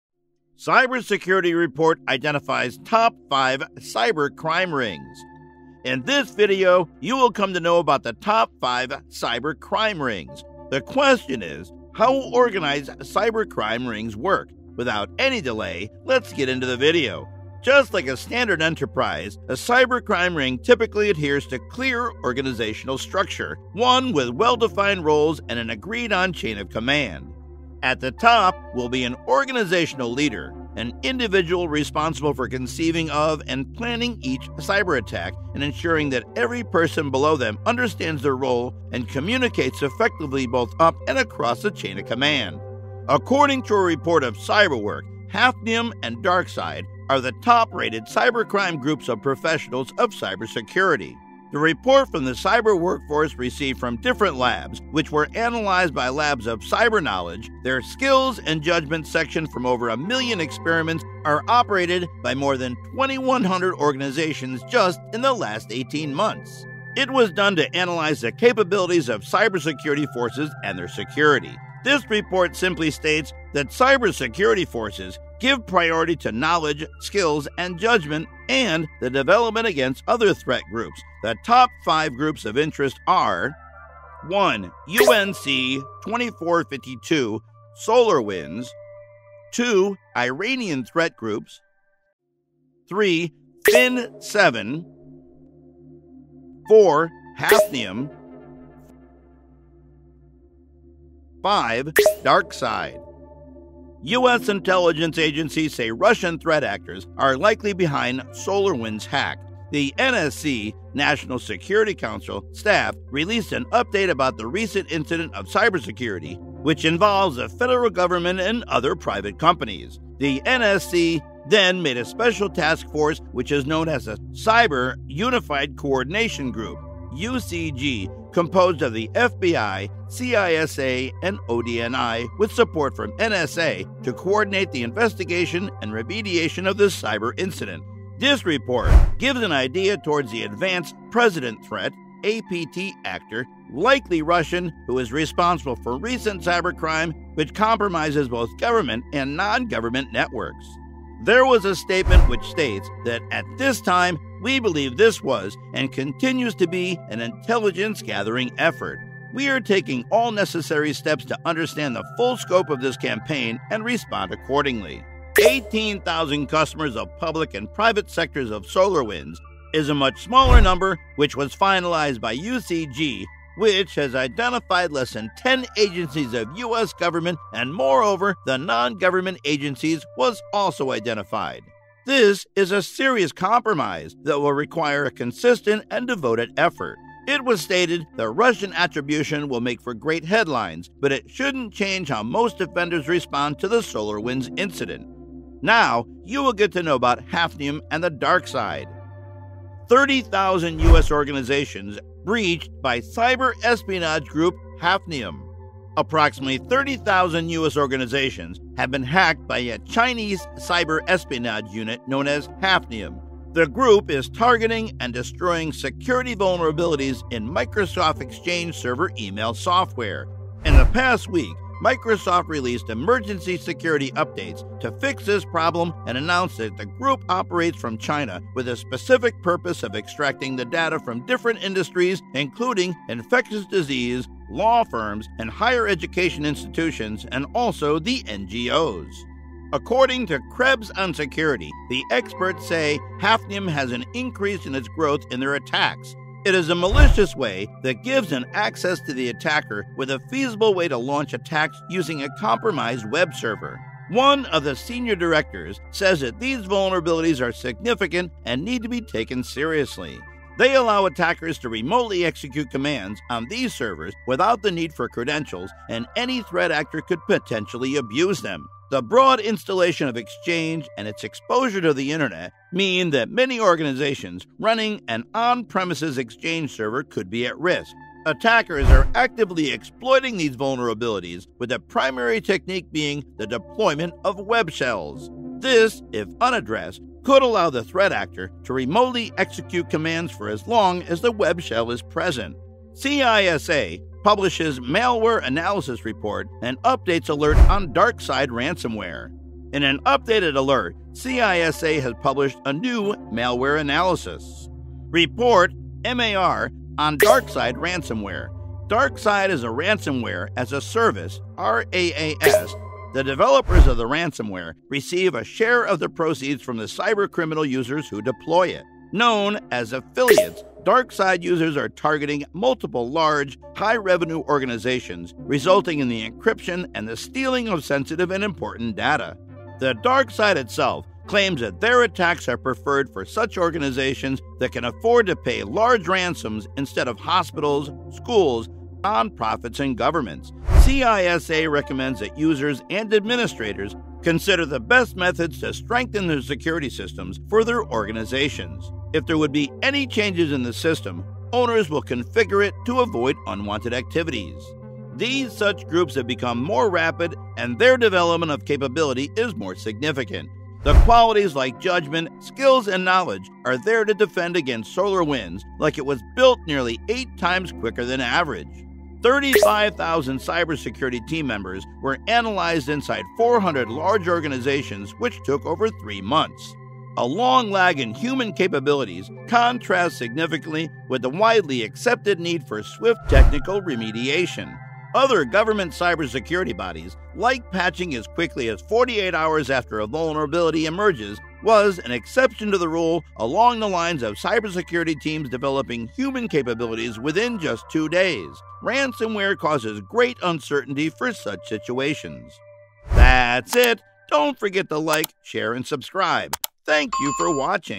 Cybersecurity Report Identifies Top 5 Cyber Crime Rings
🔴 The Top 5 Cybercrime Rings That Are Shaking the World!
Ever wondered how the world’s most dangerous cybercrime groups operate? These aren’t just lone hackers in a dark room—these are highly organized, corporate-like syndicates running sophisticated cyberattacks worldwide.

In this video, we expose the top 5 cybercrime rings and reveal their chilling methods of hacking into governments, corporations, and critical infrastructures.

🚨 Who Are These Cybercrime Rings?
🔹 UNC2452 (SolarWinds) – The supply chain attack that rocked the U.S.
🔹 Fin7 – The billion-dollar hacking cartel.
🔹 Hafnium – The espionage group behind 30,000 US breaches.
🔹 DarkSide – The ransomware gang that shut down an entire pipeline.

💀 How Do These Cybercrime Rings Work?
Just like a corporation, these cybercrime organizations have:
✅ A clear hierarchy with leaders, operators, and specialists.
✅ Sophisticated hacking tools to infiltrate even the most secure networks.
✅ Money laundering strategies that make stolen data untraceable.
✅ Connections to nation-states that use them for cyber warfare.

🔥 The Cyber Attacks That Shocked the World!
⚡ SolarWinds Breach – Russian-backed hackers infiltrated U.S. government agencies.
⚡ Hafnium Espionage – A Chinese-backed group that hacked into 30,000 U.S. organizations.
⚡ DarkSide Ransomware – The attack that took down the Colonial Pipeline, causing fuel shortages across the U.S.

🔎 Why You Should Be Concerned
With cyberattacks growing by 600%, no one is safe—governments, corporations, and even YOU could be the next target. Stay informed, stay secure.